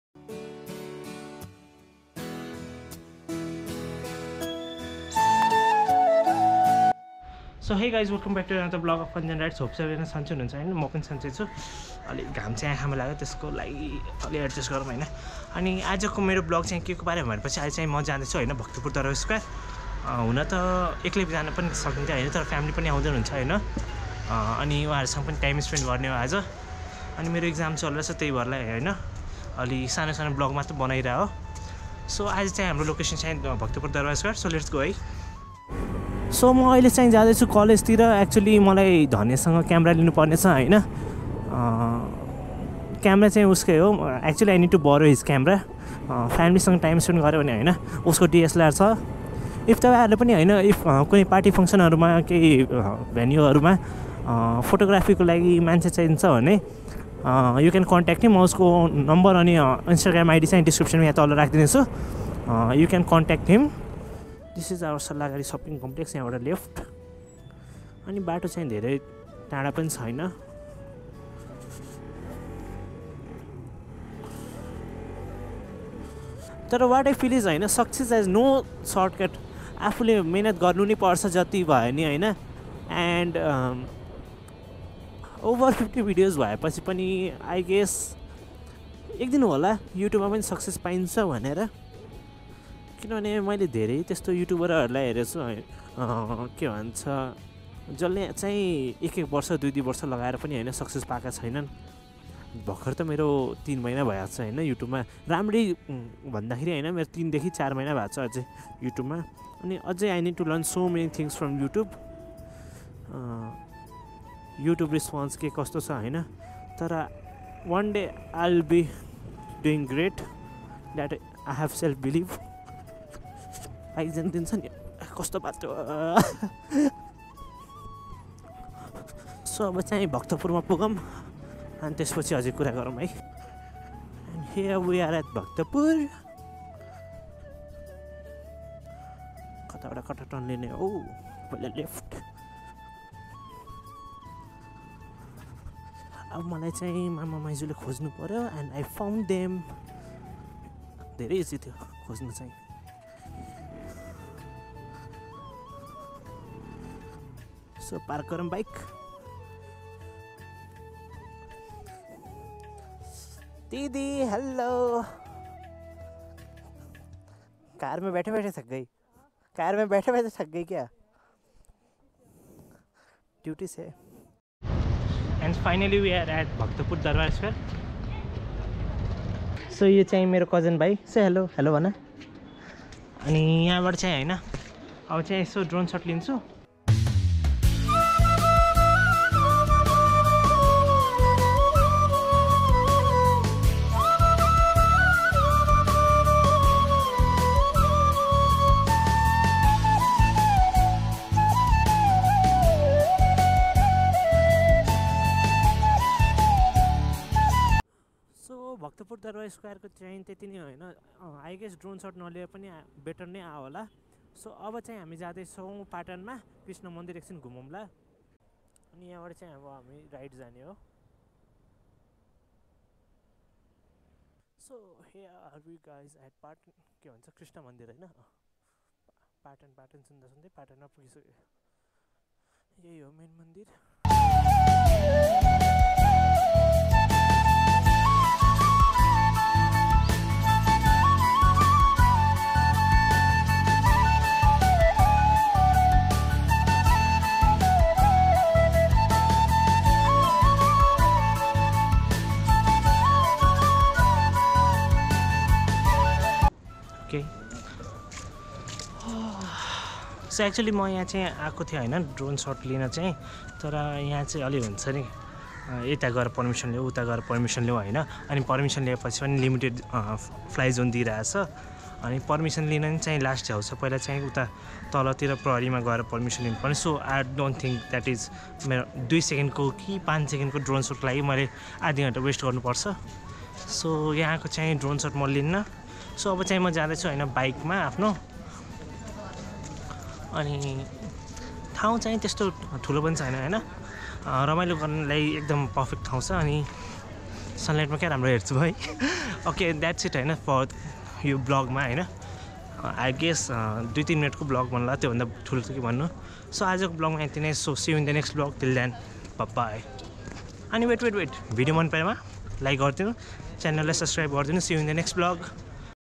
Spending Time in Bhaktapur Durbar Square | Anjan Rides
Bhaktapur, known locally as Khwopa, is a city in the east corner of the Kathmandu Valley in Nepal located about 13 kilometres from the capital city, Kathmandu. It is located in and serves as the headquarters of Bhaktapur District in Bagmati Province of Nepal. In terms of area, Bhaktapur is the smallest city of Nepal.